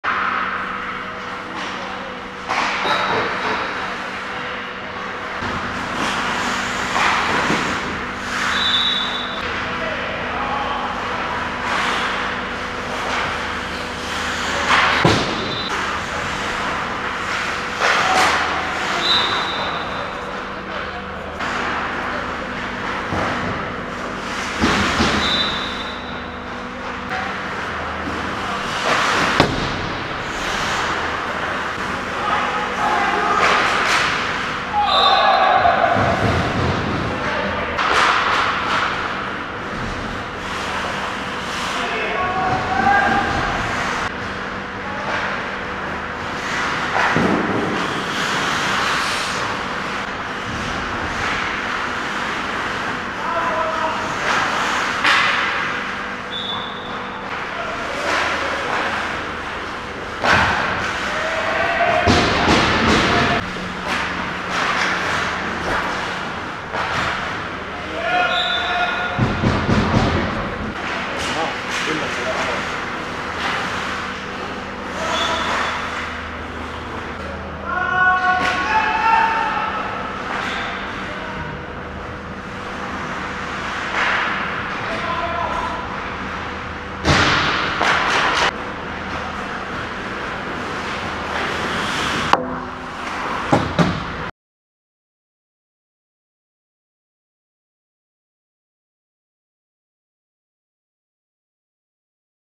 HC Rocking Cocks - Irtokiekko - goalie cam GoPro Hero3+ Black
6-1 Voitto

Oulunalueen harrastesarjaa